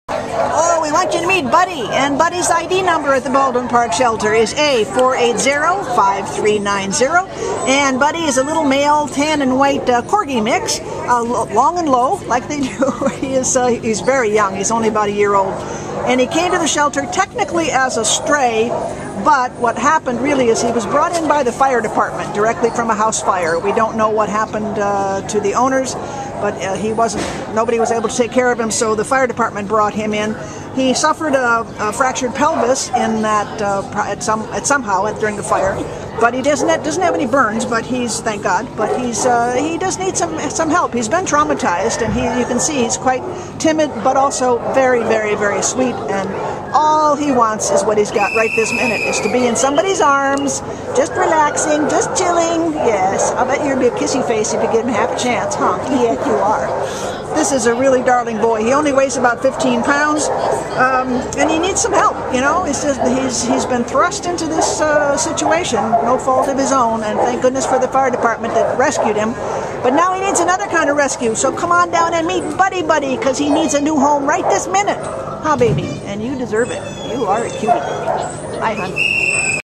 A4805390 Buddy Buddy
A4805390 Buddy Buddy is a huggy-bear 1-year-old blonde male Corgi mix who was rescued from a house fire and brought to the Baldwin Park Animal Care Center on March 4th. Weighing 15 lbs, this tiny little tank seems to have a fractured pelvis that requires cage rest. He should be seen by an outside veterinarian just to confirm. However, when our volunteers went to his kennel to get him out, he was walking around just fine, apparently unaware he is injured at all. In fact, he nearly leaped out his kennel, eager to enjoy human companionship. Buddy Buddy loves giving kisses and is immediately trusting of his handlers. It is expected that he will make an amazing and devoted indoor companion for anyone looking for a new best friend.